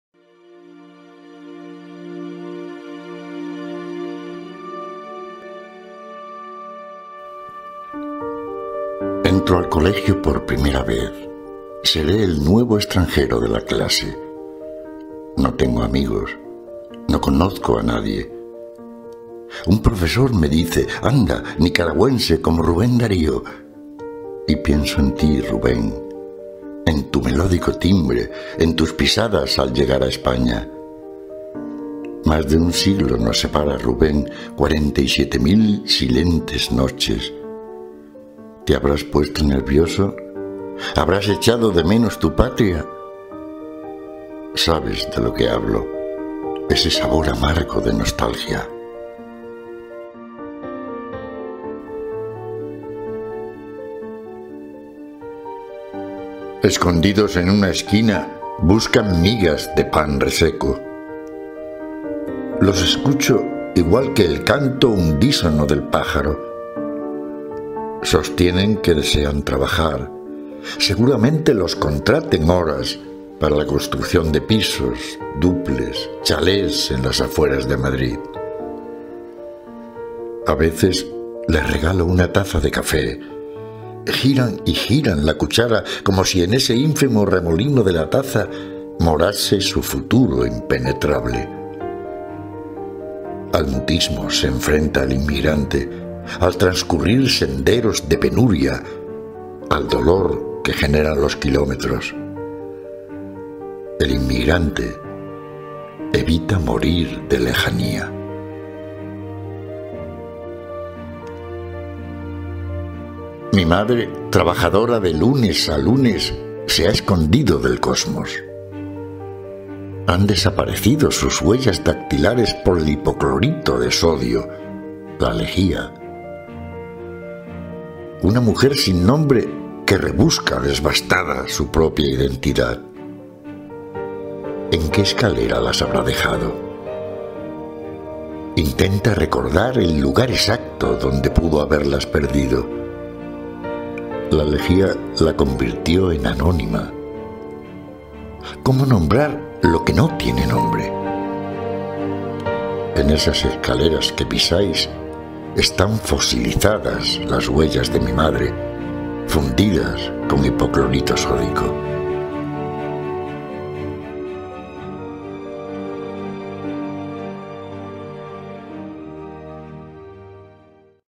BUENOS DÍAS CON .- William Alexander González Guevara
Vídeo de "3 poemas de LOS NADIES" de William Alexander González Guevara, en la voz de José Luis Rico